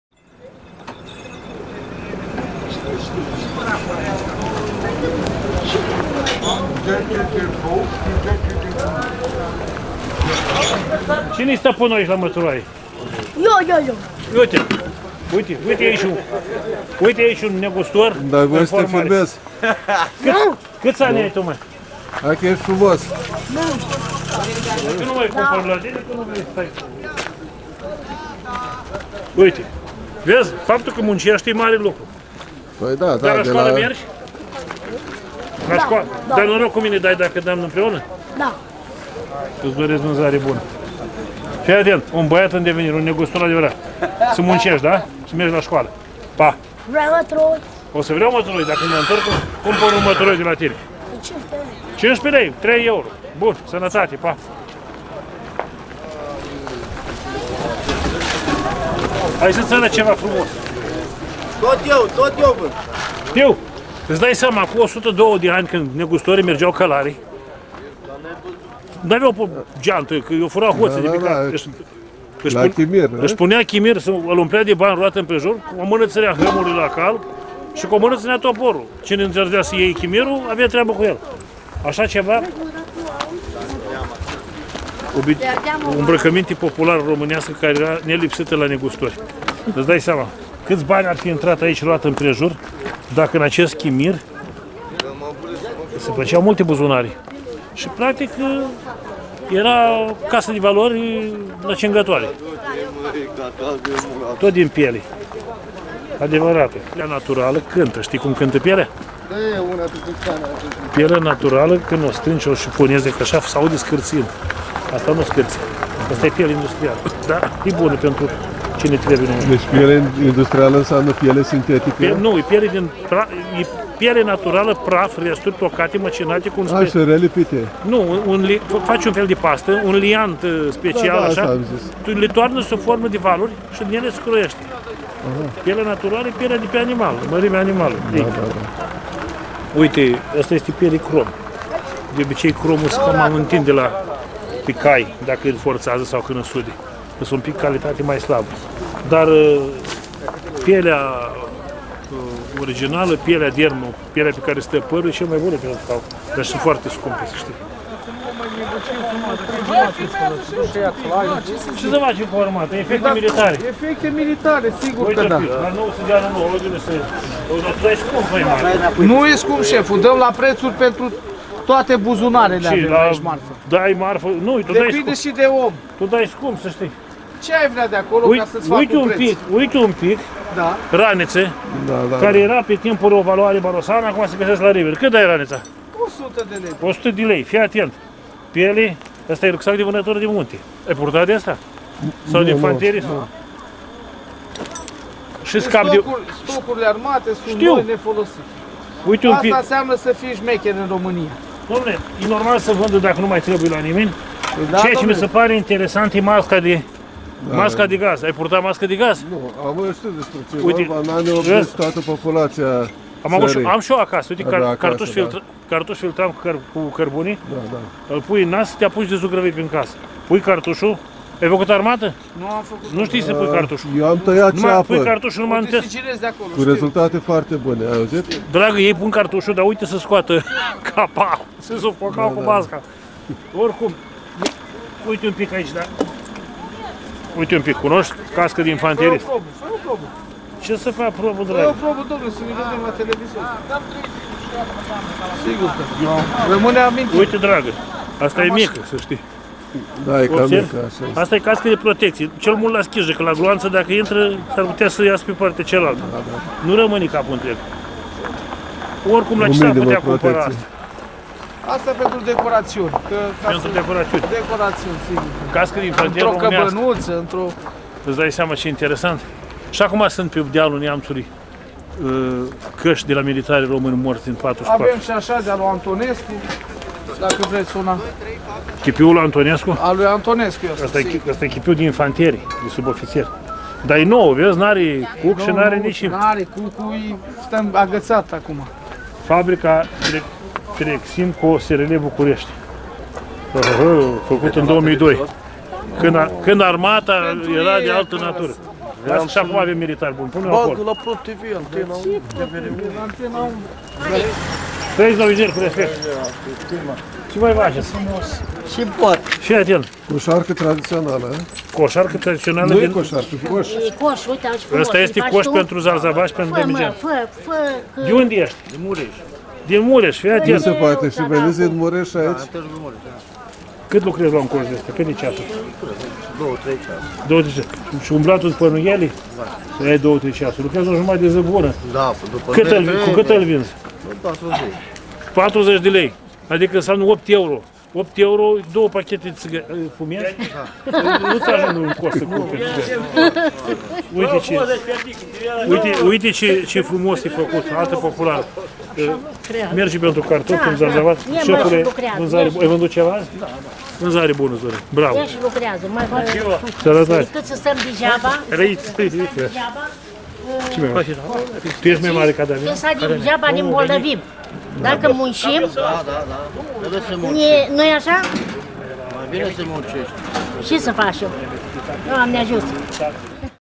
Măturoaie, chimire, efecte militare, coșuri - În oborul Humulești cu Lucian G. (Tg.Neamț, România)
Vă rog să activați subtitrarea în românește! - Please turn ON English subtitle!
Acest episod este, de asemenea, filmat la început de decembrie 2023 în oborul din Humulești. Acest obor (însemnând târg de animale, dar semănând mai mult a bazar, prezența animalelor fiind doar simbolică) este continuatorul străvechilor târguri (săptămânale) ale orașului Târgu Neamț, târguri, care de-a lungul vremii au fost treptat mutate din perimetrul orașului, până dincolo de Ozana, în satul Humulești, vestit pentru „Amintirile din copilărie” ale lui marelui povestitor Ion Creangă. De fapt, Ion Creangă a încercat să vândă o pupăză în târgul care la acea vreme era amplasat la poalele Cetății Neamț.
În acest episod facem cunoștință cu mărfurile a patru vânzători, mărfuri care au în comun faptul că nu sunt comestibile. Pentru început intrăm în discuție cu un puști foarte pitoresc, puțin „sălbatic”, care, cumva, vindea măturoaie. Lucian Georgescu îl laudă pentru istețimea sa și îl îndeamnă să nu renunțe la școală. Privirea lui Lucian Georgescu, mare iubitor de cai și cunoscător al lor și al harnașamentului este atrasă de alte articole mai speciale din piele și anume chimirele tradiționale. Acestea sunt vândute, cumva curios, tot de țiganul căldărar din zona Pașcani, cel care producea și vindea într-un epidod anterior cazane de rachiu și oale de tuci În vecinătatea acestuia, probabil tot țigan din aceeași zonă, vinde obiecte mai rare în târguri și anume efecte militare, lucruri noi rezultate din stocurile depășite, clasate, ale armatei. „Asta înseamnă să fii șmecher în România!” se laudă domnul respectiv. La ieșirea din obor, în timp ce discutam cu o doamnă din Humulești despre minunățiile de covoare tradiționale țesute manual Lucian Georgescu „agață” un producător-vânzător de coșuri tradiționale împletite din nuiele, venit și el de departe, tocmai din Târgu Mureș.